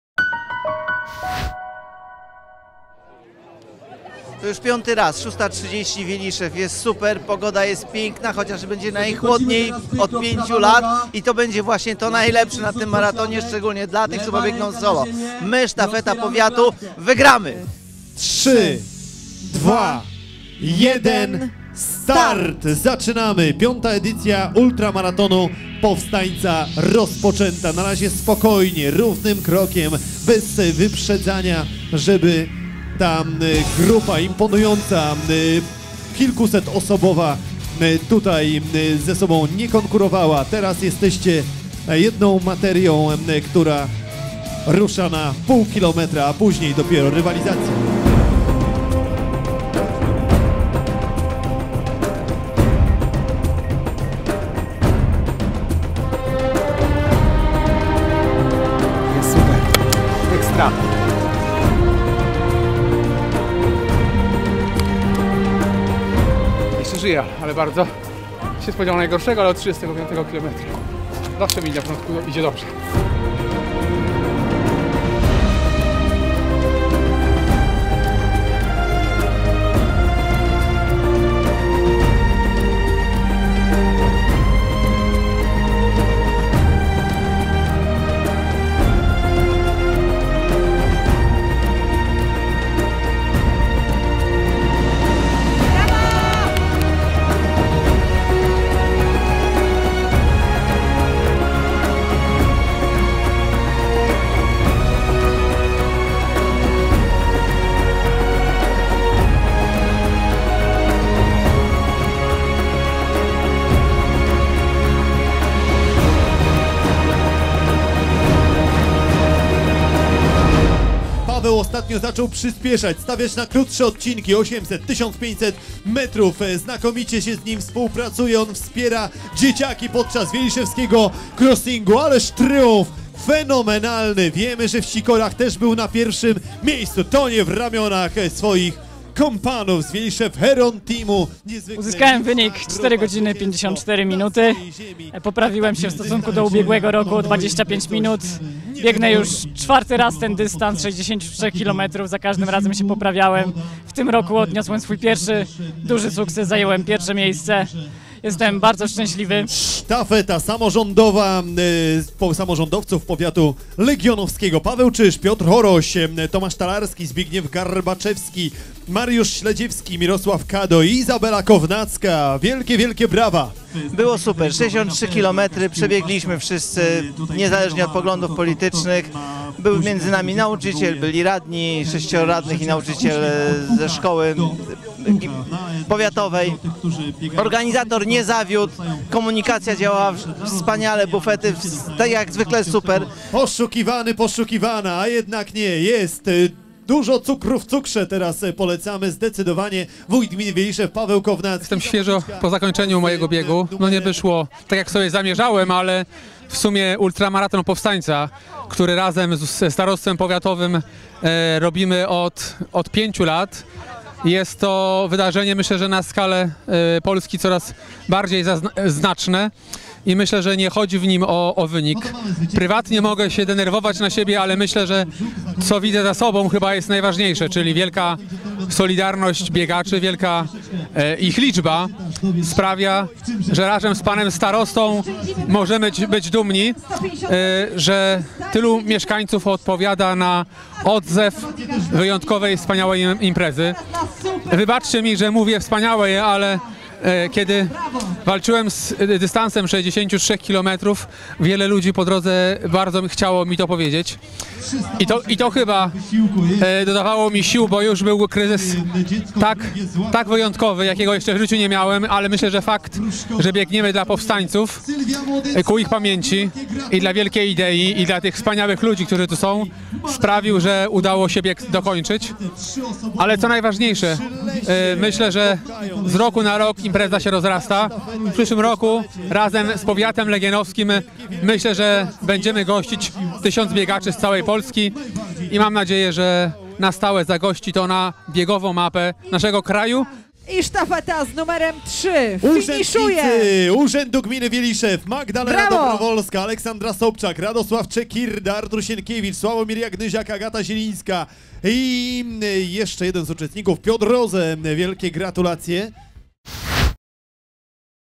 5. Ultramaraton Powstańca
4 sierpnia br. w Gmine Wieliszew już po raz piąty odbył się Ultramaraton Powstańca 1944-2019. Kilkuset biegaczy wzięło udział w wydarzeniu, które nie tylko dostarcza sportowych wyzwań i emocji, ale przede wszystkim integruje nas wokół pamięci o Powstańcach Warszawskich.